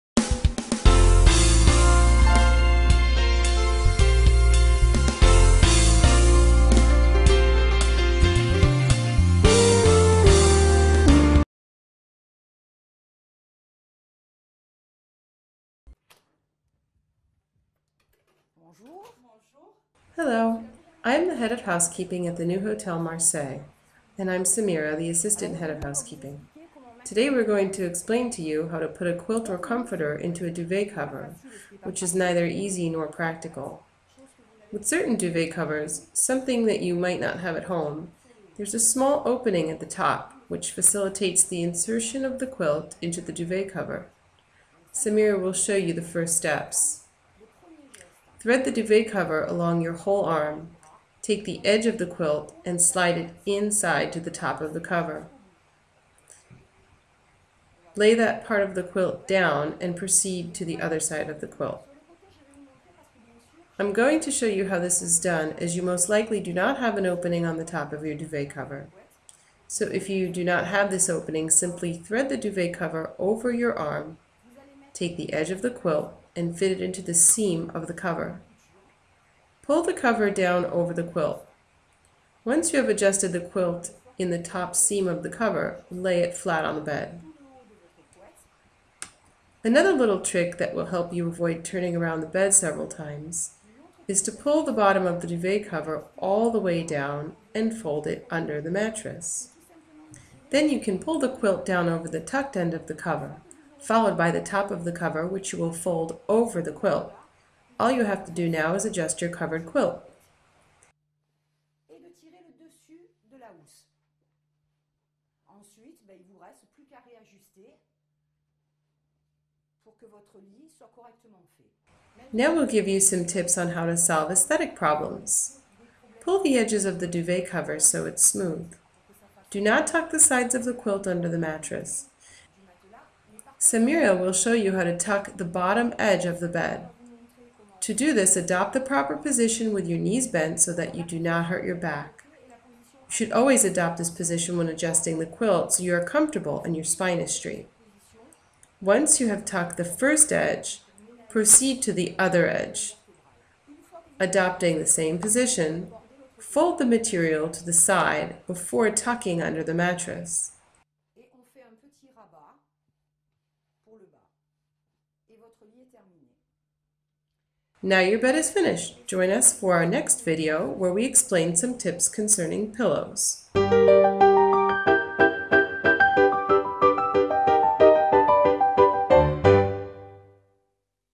How to Put a Duvet Cover
Safer, head housekeeper at the New Hotel of Marseille and her assistant Samira teach you how to put a duvet cover easily. Thanks to them, this delicate operation is nothing but a bad memory...